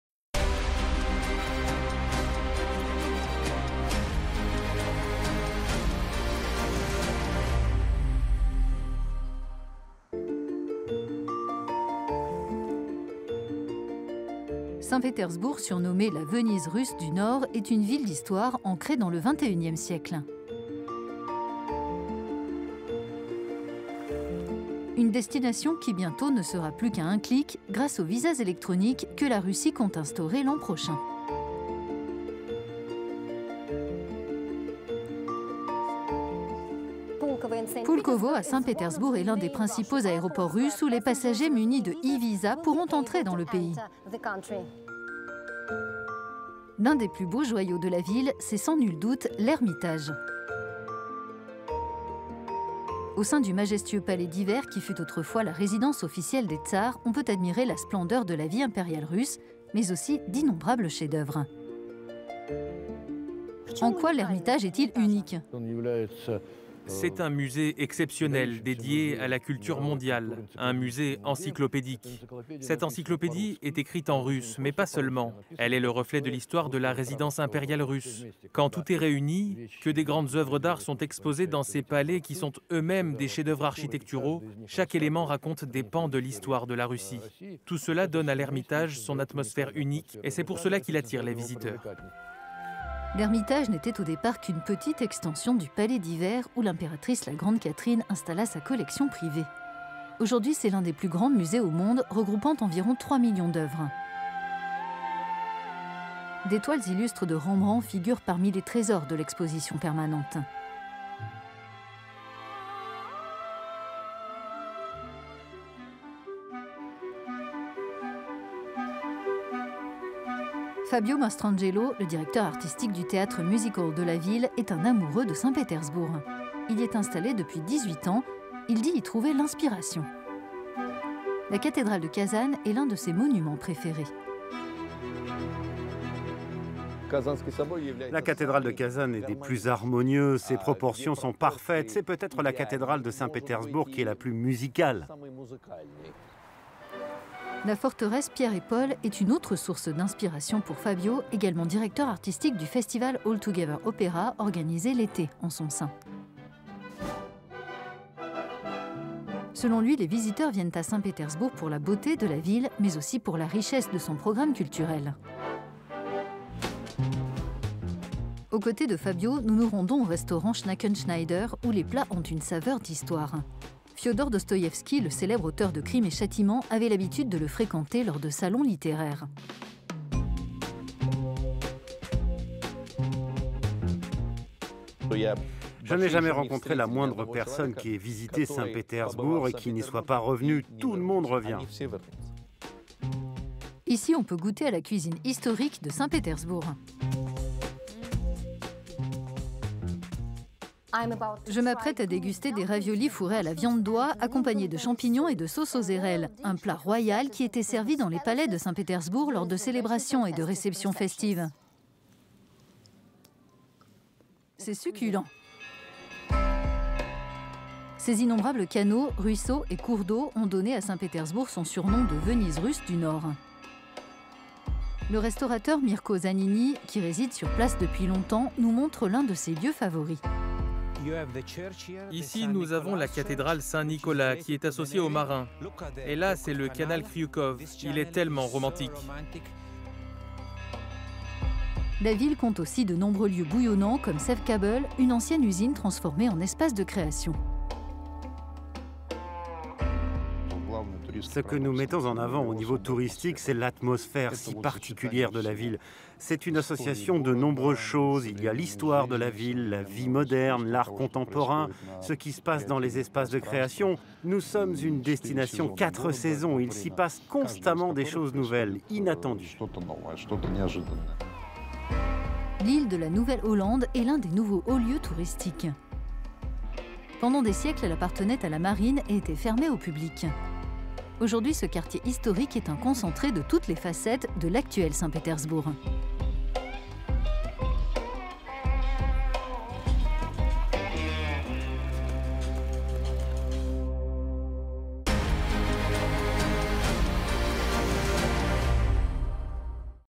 Saint-Pétersbourg entre splendeur du passé et bouillonnement du présent
​Avec ses chefs-d'œuvre réunis par les empereurs russes et ses merveilles architecturales, Saint-Pétersbourg est surnommée "la Venise russe du Nord". Nous passons en revue ses attraits anciens et modernes.